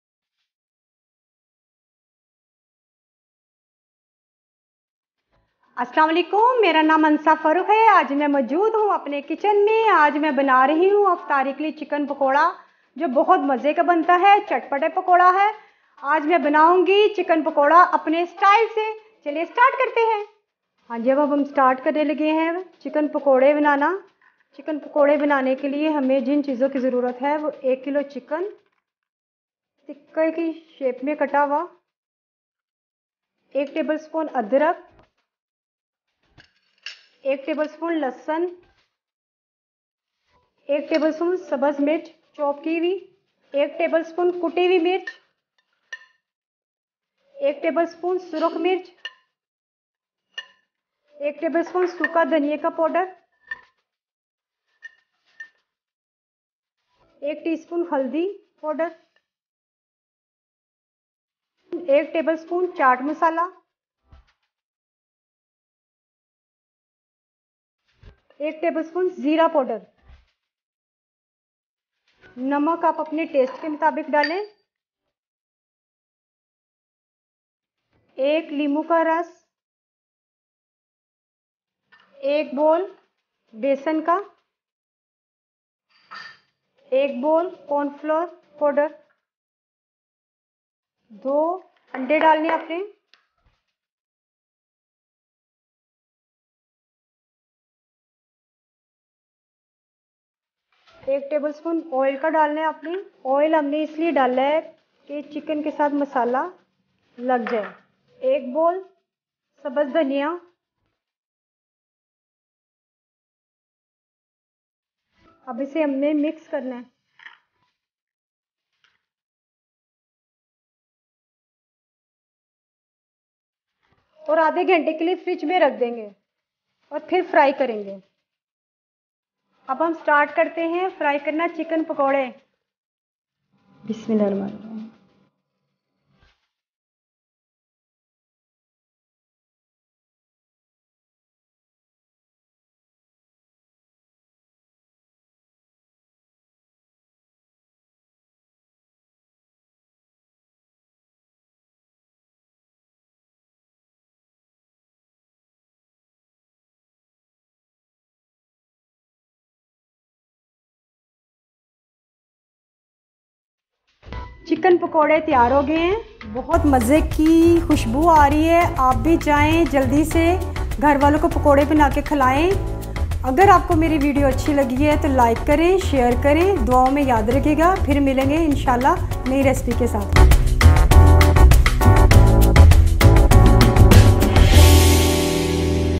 Chicken pakoray | Ramadan special recipe
Ramadan Special! Chicken Pakora Recipe a Special Ramadan Recipe by Kitchen With Khansa. A Delicious Chicken Pakora Recipe for iftar time in Ramadan. Super Taste Pakora or Pakoda Recipe by Khansa.